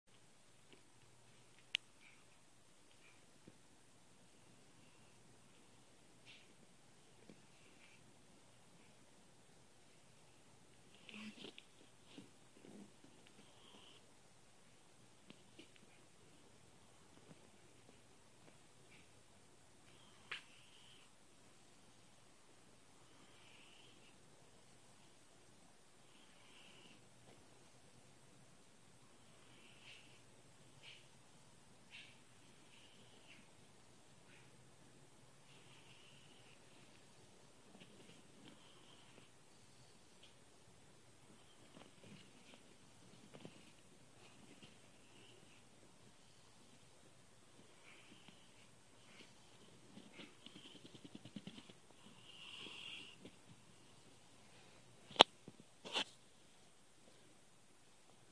Cat and Rat Friendship (II)
Petri the cat and Angelica the rat, circa 2014.